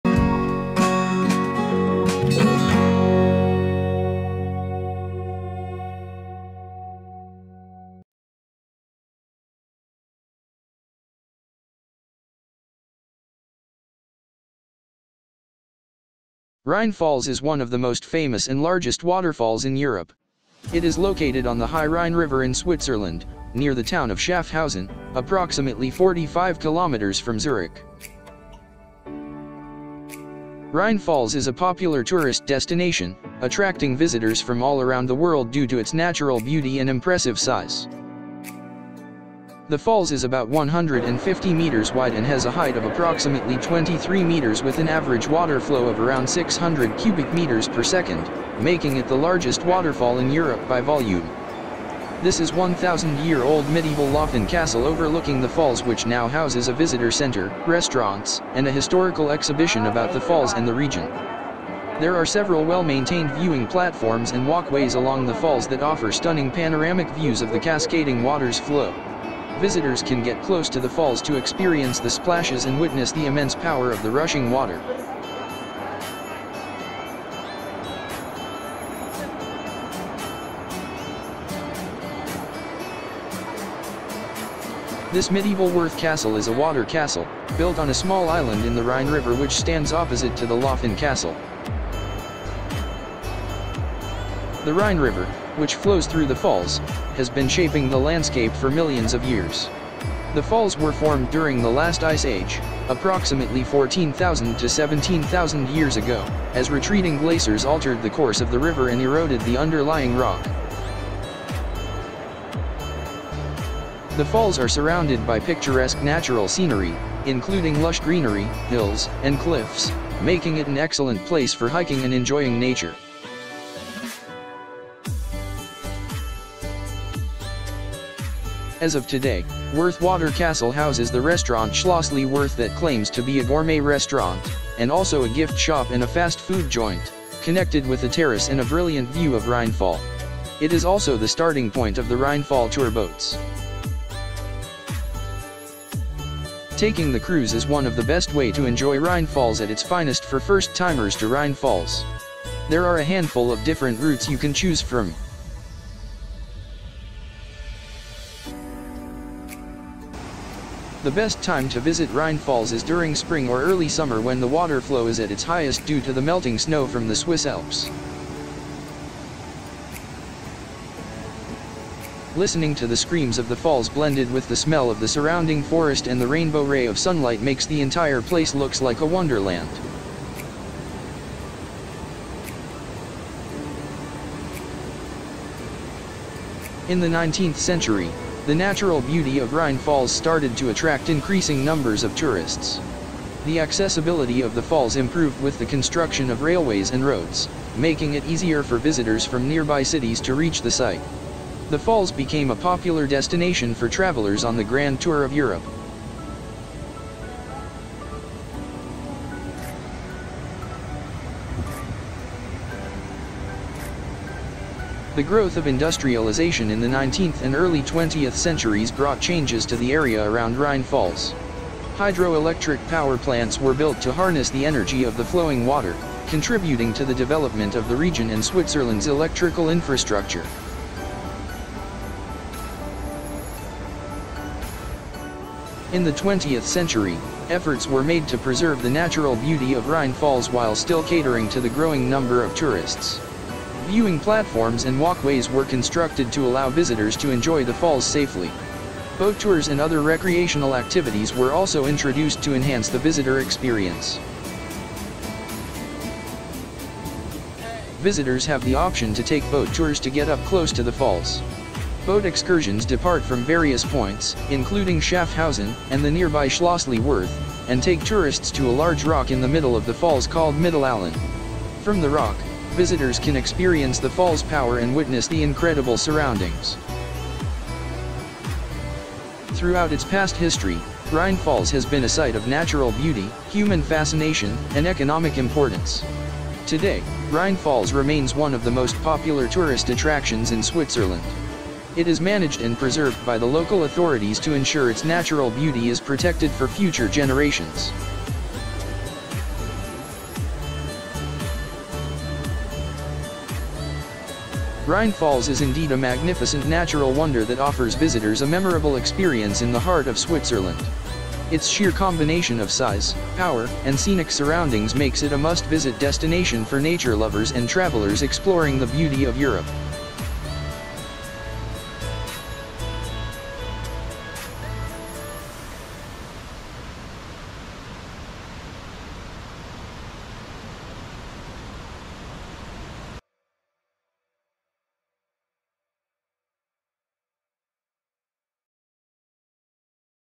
A Day Trip to Picturesque Rhine Falls Europe's Largest Falls Switzerland Magnificent Wonders
Rhine Falls, also known as Rheinfall in German, is one of the most famous and largest waterfalls in Europe. It is located on the High Rhine River in Switzerland, near the town of Schaffhausen, approximately 45 kilometers from Zurich. Rhine Falls is a popular tourist destination, attracting visitors from all around the world due to its natural beauty and impressive size.

The best time to visit Rhine Falls is during spring or early summer when the water flow is at its highest due to the melting snow from the Swiss Alps. However, the falls are impressive throughout the year.

A Day Trip to Rhine Falls Europe Largest Falls Schaffhausen Switzerland Magnificent Wonders